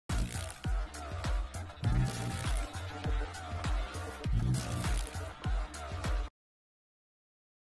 robot bird：smallest robot bee
This is the world's smallest robotic bee, developed by a university research institute in India?